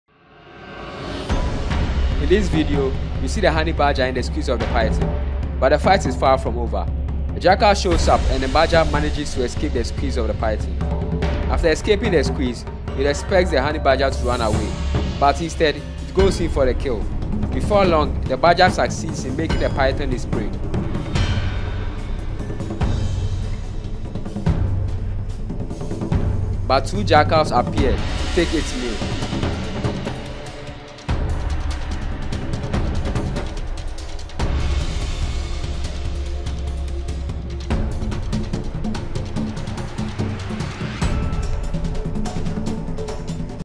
HONEY BADGER ESCAPES THE SQUEEZE OF THE PYTHON BUT WHAT HAPPENS NEXT WILL SHOCK YOU😱😨
The honey badger is a fierce animal and no matter the enemy it confronts it still stands firm. In this video, we see the honey badger in action against this big python and the two jackals